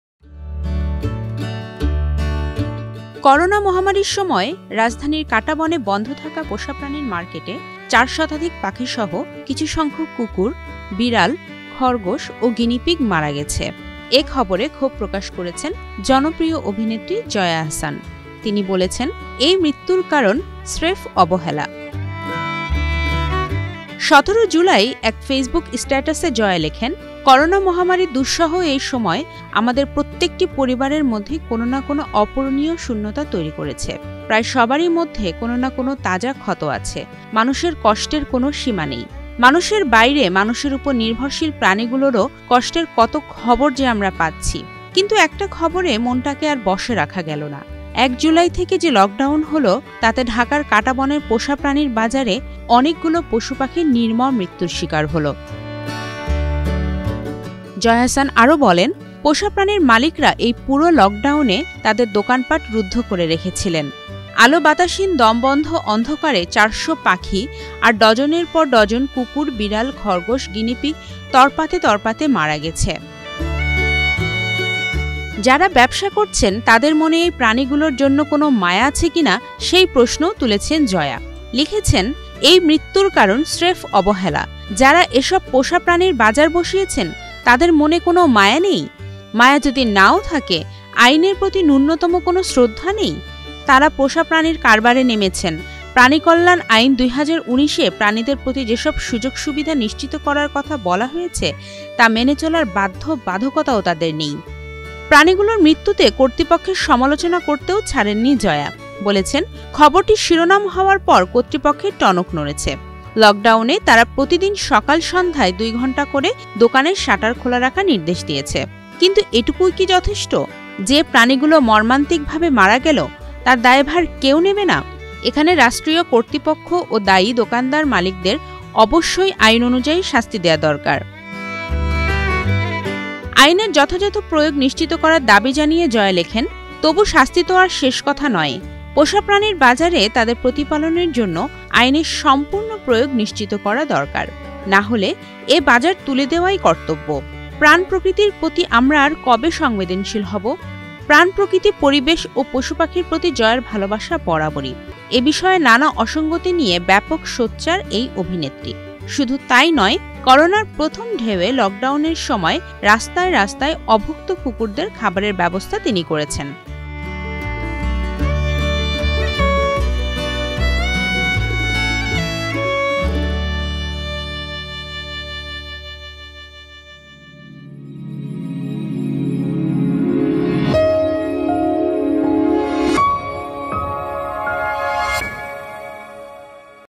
প্রাণীদের জন্য সোচ্চার জয়া | Jaya Ahsan | দেশ রূপান্তর
সম্প্রতি গণমাধ্যমে খবর বেরিয়েছে, করোনা মহামারির সময় রাজধানীর কাঁটাবনে পোষা প্রাণীর মার্কেটে চার শতাধিক পাখিসহ কিছুসংখ্যক কুকুর, বিড়াল, খরগোশ ও গিনিপিগ মারা গেছে। এ খবরে ক্ষোভ প্রকাশ করেছেন দুই বাংলার জনপ্রিয় অভিনেত্রী জয়া আহসান। বলেছেন, ‘এই মৃত্যুর কারণ স্রেফ অবহেলা।’
১৭ জুলাই এক ফেসবুক স্ট্যাটাসে জয়া লেখেন, ‘করোনা মহামারির দুঃসহ এই সময় আমাদের প্রত্যেকটি পরিবারের মধ্যেই কোনো না কোনো অপূরণীয় শূন্যতা তৈরি করেছে। প্রায় সবারই মধ্যে কোনো না কোনো তাজা ক্ষত আছে। মানুষের কষ্টের কোনো সীমা নেই।
‘মানুষের বাইরে মানুষের ওপর নির্ভরশীল প্রাণীগুলোরও কষ্টের কত খবর যে আমরা পাচ্ছি। কিন্তু একটা খবরে মনটাকে আর বশে রাখা গেল না। ১ জুলাই থেকে যে লকডাউন হলো, তাতে ঢাকার কাঁটাবনের পোষা প্রাণীর বাজারে অনেকগুলো পশুপাখি নির্মম মৃত্যুর শিকার হলো।’
জয়া আহসান বলেন, ‘পোষা প্রাণীর মালিকরা এই পুরো লকডাউনে তাদের দোকানপাট রুদ্ধ করে রেখেছিলেন। আলো-বাতাসহীন দমবন্ধ অন্ধকারে ৪০০ পাখি আর ডজনের পর ডজন কুকুর, বিড়াল, খরগোশ, গিনিপিগ তড়পাতে তড়পাতে মারা গেছে।’
যারা ব্যবসা করছেন তাদের মনে এই প্রাণীগুলোর জন্য কোনো মায়া আছে কি না, সেই প্রশ্নও তুলেছেন জয়া। লেখেন, “এই মৃত্যুর কারণ স্রেফ অবহেলা। যারা এসব পোষা প্রাণীর বাজার বসিয়েছেন, তাদের মনে মায়া নেই? মায়া যদি না-ও থাকে, আইনের প্রতি ন্যূনতম কোনো শ্রদ্ধা নেই? তারা পোষা প্রাণীর কারবারে নেমেছেন, ‘প্রাণীকল্যাণ আইন ২০১৯’-এ প্রাণীদের প্রতি যেসব সুযোগ-সুবিধা নিশ্চিত করার কথা বলা হয়েছে, তা মেনে চলার বাধ্যবাধকতা তাদের নেই?”
প্রাণীগুলোর মৃত্যুতে কর্তৃপক্ষের সমালোচনা করতেও ছাড়েননি জয়া। বলেছেন, ‘খবরটি শিরোনাম হওয়ার পর কর্তৃপক্ষের টনক নড়েছে। লকডাউনে তারা প্রতিদিন সকালে-সন্ধ্যায় দুই ঘণ্টা করে দোকানের শাটার খোলা রাখার আদেশ দিয়েছে।
‘কিন্তু এটুকুই কি যথেষ্ট? যে প্রাণীগুলো মর্মান্তিকভাবে মারা গেল, তার দায়ভার কেউ নেবে না? এখানে রাষ্ট্রীয় কর্তৃপক্ষ ও দায়ী দোকান মালিকদের অবশ্যই আইন অনুযায়ী শাস্তি দেয়া দরকার।’
আইনের যথাযথ প্রয়োগ নিশ্চিত করার দাবি জানিয়ে জয়া লেখেন, ‘তবু শাস্তি তো আর শেষ কথা নয়। পোষা প্রাণীর বাজারে তাদের প্রতিপালনের জন্য আইনের সম্পূর্ণ প্রয়োগ নিশ্চিত করা দরকার। না হলে এ বাজার তুলে দেয়াই কর্তব্য। প্রাণ-প্রকৃতির প্রতি আমরা আর কবে সংবেদনশীল হব!’
প্রাণ-প্রকৃতি, পরিবেশ ও পশুপাখির প্রতি জয়ার ভালোবাসা বরাবরই। এসব বিষয়ে নানা অসংগতি নিয়ে ব্যাপক সোচ্চার এই অভিনেত্রী। শুধু তা-ই নয়, করোনার প্রথম ঢেউয়ে লকডাউনের সময় রাস্তায় রাস্তায় অভুক্ত কুকুরদের খাবারের ব্যবস্থা করেন তিনি।